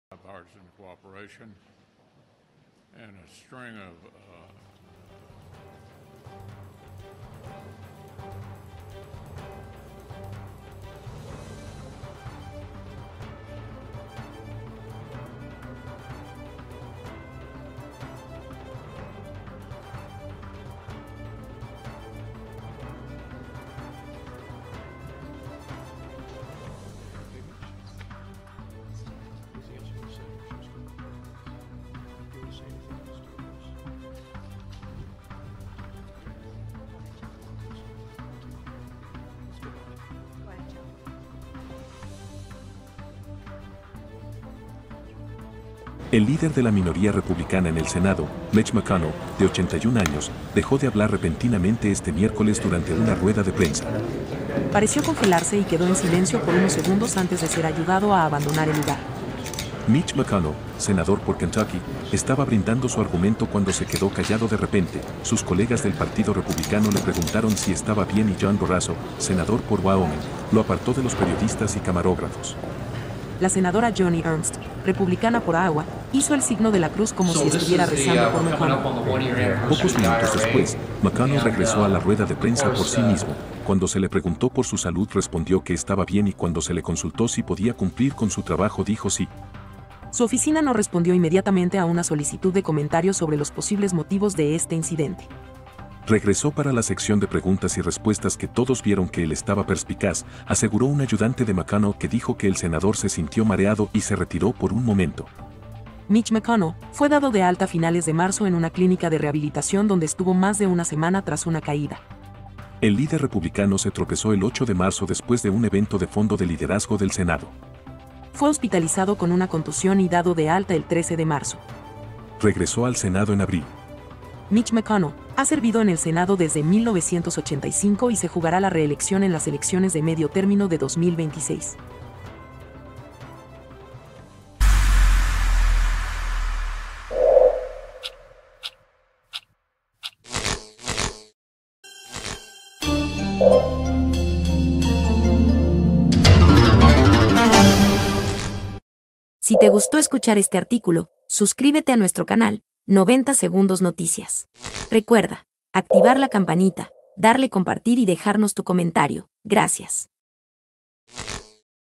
🛑El líder de los republicanos en el Senado de EEUU McConnell se queda 'congelado' en rueda de prensa
El líder de la minoría republicana en el Senado, Mitch McConnell, de 81 años, dejó de hablar repentinamente este miércoles durante una rueda de prensa, pareció congelarse y quedó en silencio por unos segundos antes de ser ayudado a abandonar el lugar.

McConnell, senador por Kentucky, estaba brindando su argumento cuando se quedó callado de repente; sus colegas del Partido Republicano le preguntaron si estaba bien y John Barrasso, senador por Wyoming, lo apartó de los periodistas y camarógrafos.
Republican Senate Minority Leader Mitch McConnell, 81, suddenly stopped speaking during a news conference on Wednesday, appearing to freeze and remain silent for a few seconds before being helped out.

McConnell, a senator from Kentucky, was making his point when he suddenly fell silent; his GOP colleagues asked him if he was okay, and John Barrasso, a Wyoming senator, pulled him away from reporters and cameramen.

90 Segundos Noticias, Somos independientes, con neutralidad e imparciales, objetivos y honestos en su concepto estricto. Sin prejuicios, justos y equilibrados. ...